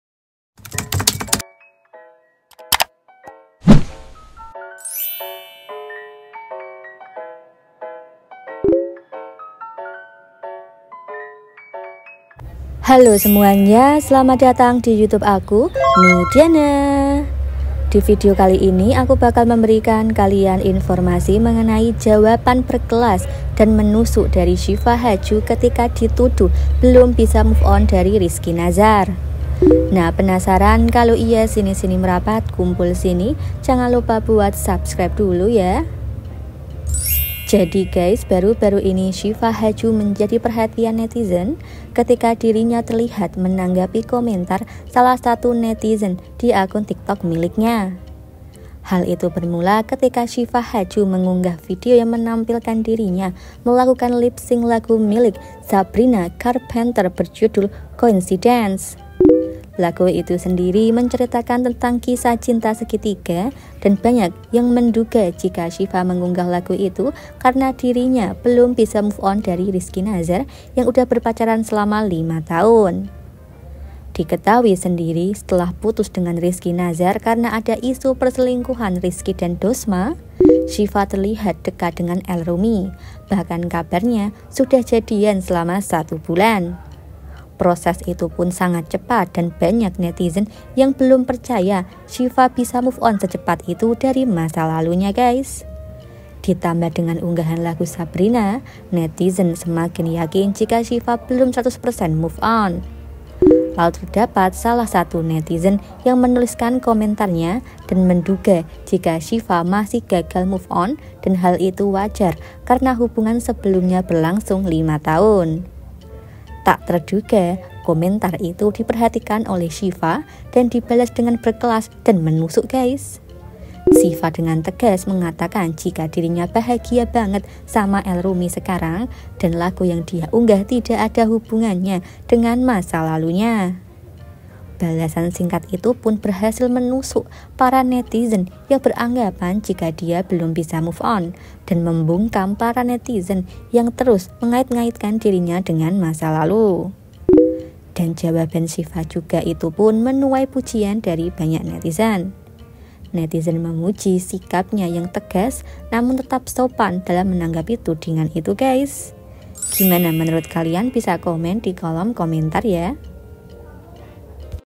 SYIFA HADJU JAWAB HATTERS YANG SANGKUTKAN DENGAN RIZKY NAZAR, UDAH MASA LALU WOY!!
SYIFA HADJU JAWAB HATTERS YANG SANGKUTKAN DIA DENGAN RIZKY NAZAR